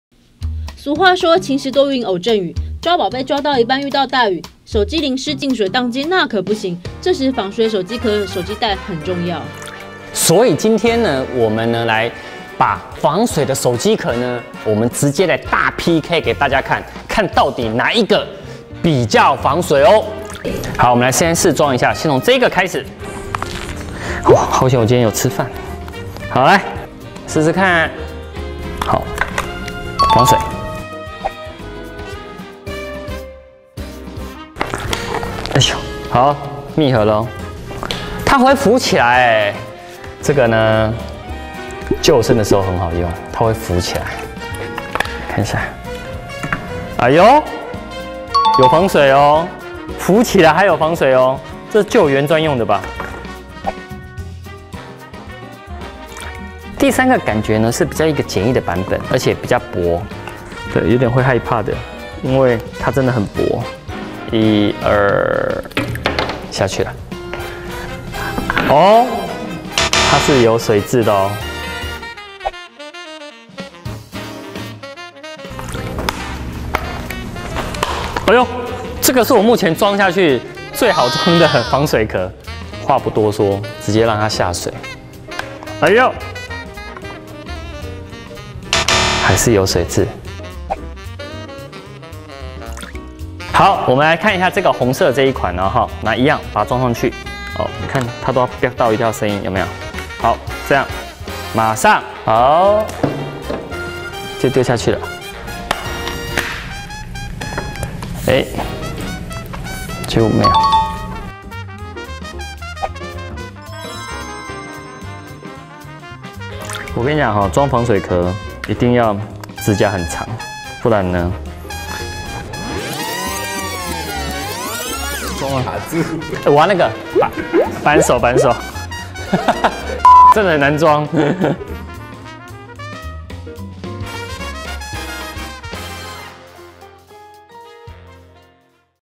【台灣壹週刊】抓寶不怕濕 防水手機殼評比
俗話說晴時多雨偶陣雨，遇到突如其來的雨勢，該怎麼保護手機不進水？畢竟，下雨也得抓寶啊！而且現在戶外活動這麼流行，手機很容易碰到水。本刊集合市面買得到的防水手機殼與袋做評比，那個可讓你雨天也能安心抓寶收怪，看完便知。